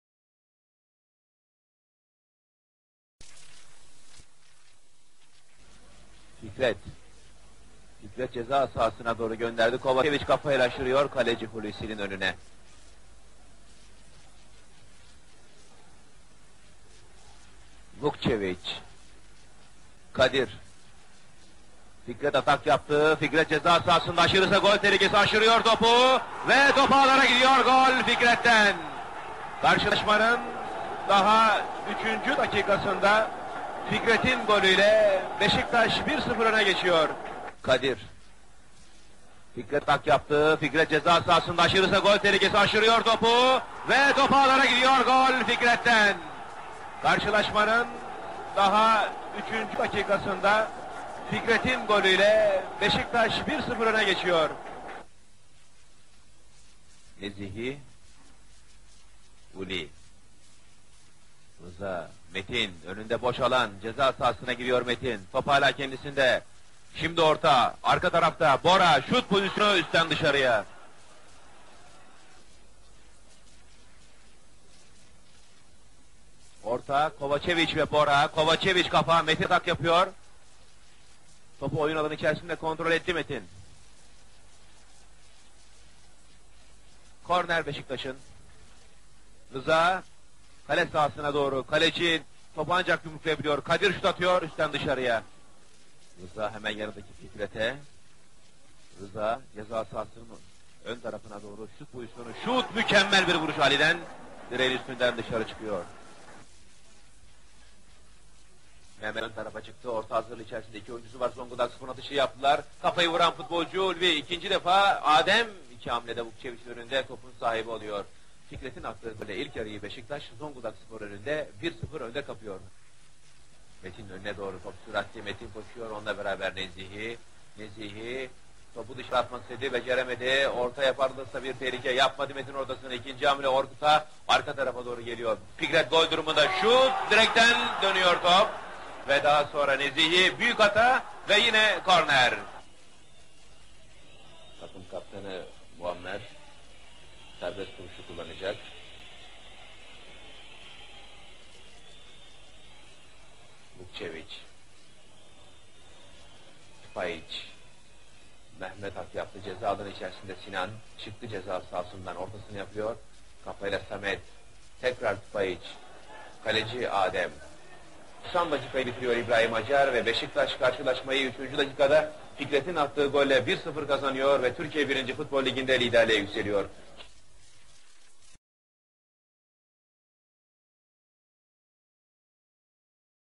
1985-86 Sezonu 26.01.1986 Beşiktaş – Zonguldakspor:1-0
13.755 biletli seyircinin izlediği karşılaşma çok ağır bir sahada oynandı. Futbol değil sanki su topu maçı oldu.
Szager (Zonguldakspor Antrenörü): ‘Yenilgiyi hak etmedik. Beşiktaş, birinci yarıda üstün oynadı, ancak ikinci yarıda biz daha iyiydik. Çok gol kaçırdık.’
Mümtaz Soysal (Hukukçu): ‘Dünyanın başka herhangi bir yerinde böyle saha futbola elverişsiz ilan edilirdi ama Türkiye’nin ölçülerine göre yine de kötülerin en kötüsü sayılmazdı. Nitekim maç sonuna kadar oynandı. Yapışkan çamurun gülle gibi ağırlaştırdığı top, kah su birikintilerinde yavaş yavaş yuvarlanıyor, kah kirli kısımların bataklığına saplanıp kalıyordu. Ama 22 genç, Dolmabahçe’nin bu içler acısı durumuna aldırış etmeksizin, sonuna kadar kendilerini tüketircesine didindiler.’
Ziya Şengül (Gazeteci): ‘Bir maç ki ortaya kalite getirmesi, piyangodan milyar çıkmasını beklemek gibi bir şey. Maçın genelinde ne taktik ne de sistem aramak mümkün değil. Kaybeden Zonguldakspor’a asla sitem etmiyoruz.’